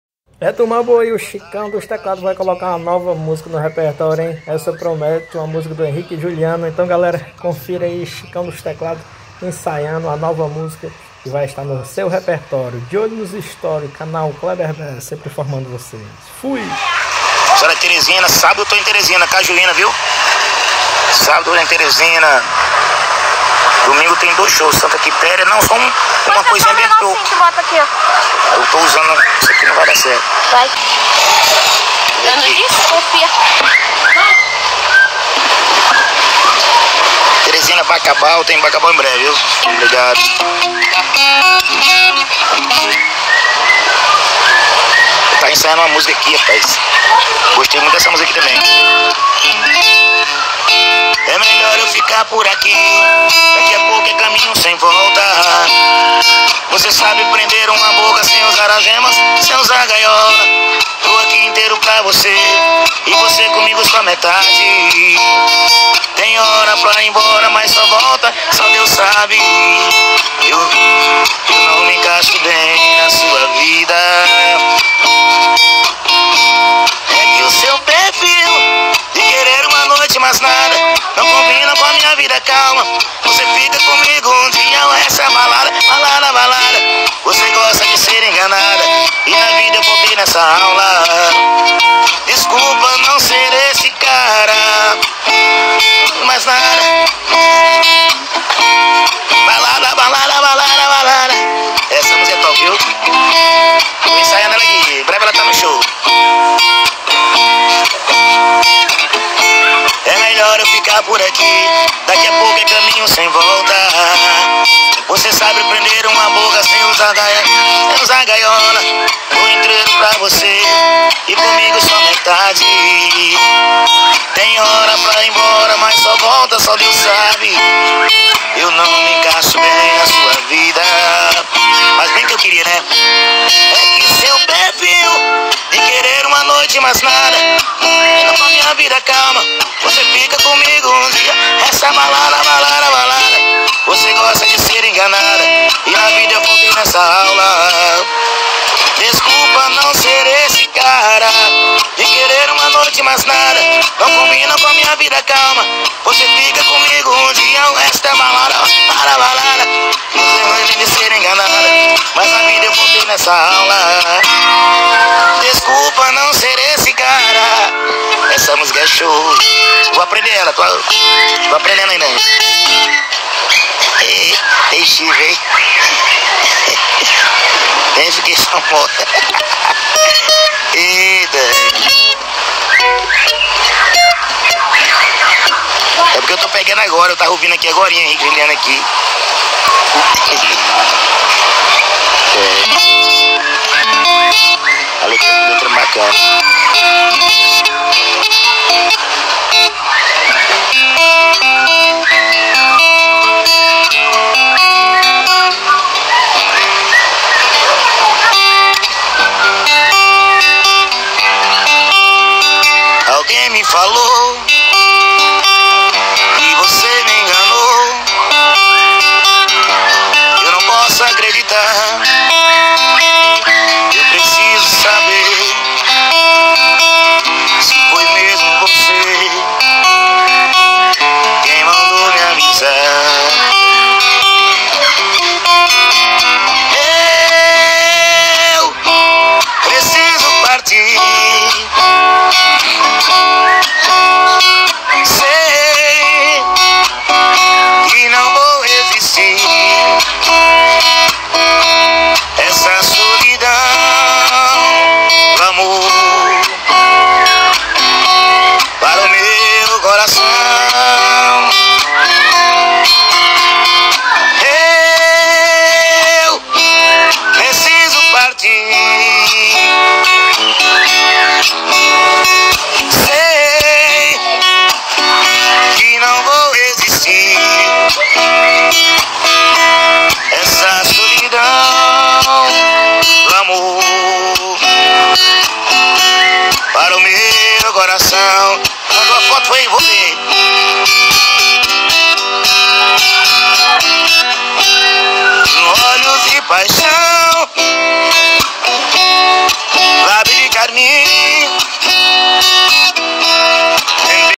VEM AI NOVA MUSICA NO REPERTÓRIO DE CHICÃO DOS TECLADOS VEJA 👀.
Chicão ensaia nova música para o repertório esse cara herique e Juliano.